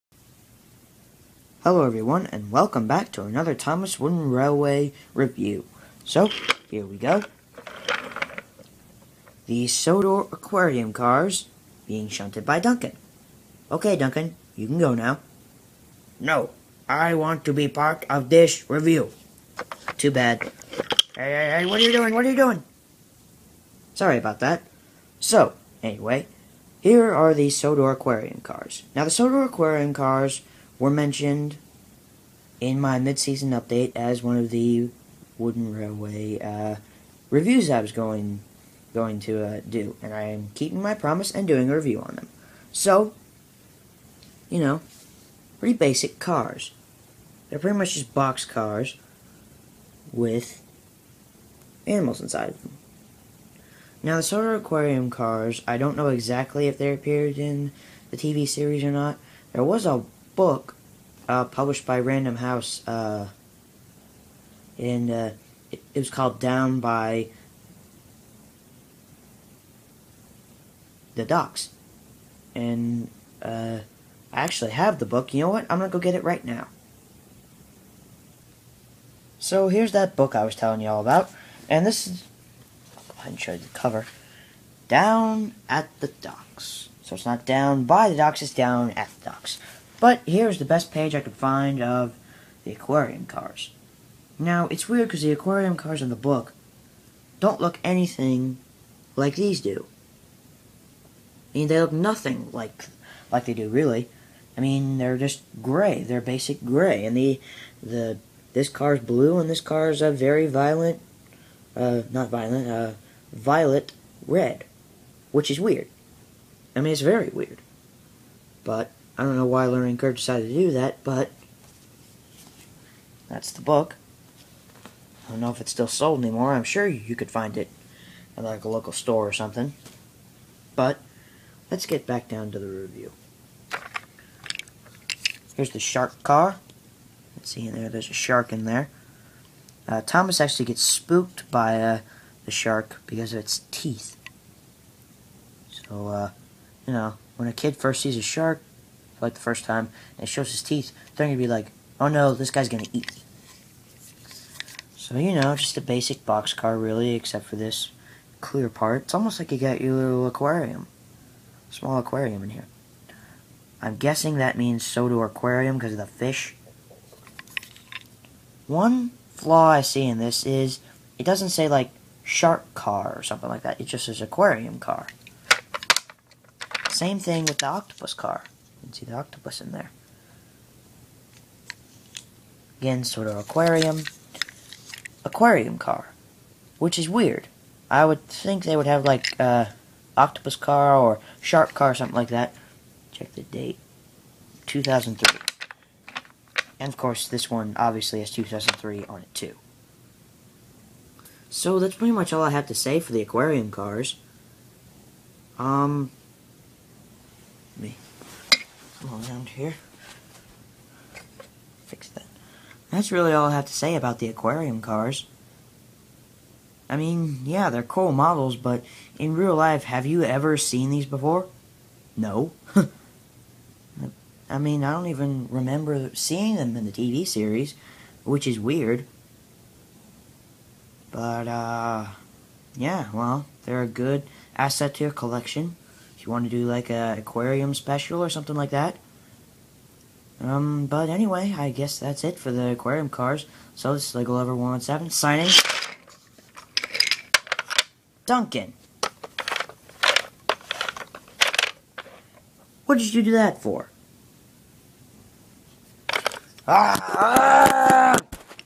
Thomas Wooden Railway Review: Aquarium Cars
A review on the aquarium cars from the Thomas Wooden Railway.